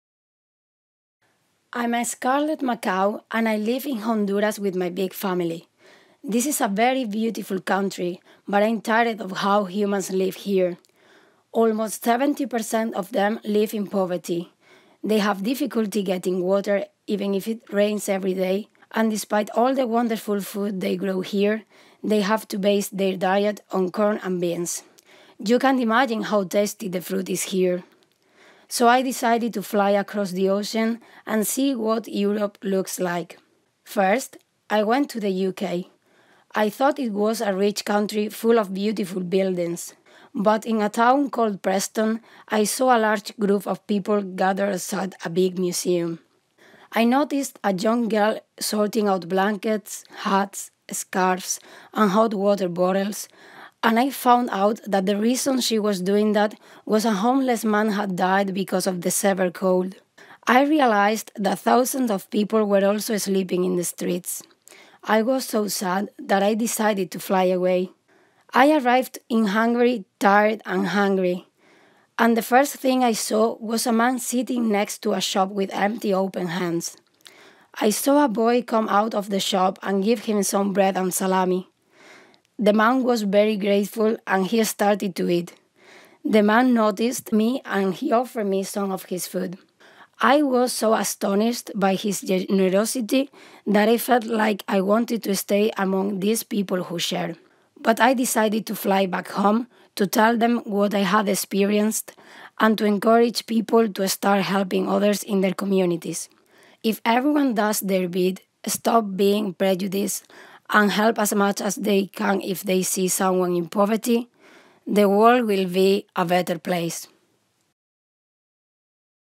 The flight of the bird
This film was made as part of the Youth in Progress and Partnership for Equality and Exchange (YIPPEE) project is a project funded under the Erasmus + Capacity Capacity Building Program which aims to promote the use of digital storytelling method as way to enhance youth’s competences and to raise awareness  among young people about Sustainable Development Goals.

This video is refers to the Goal n.1 Poverty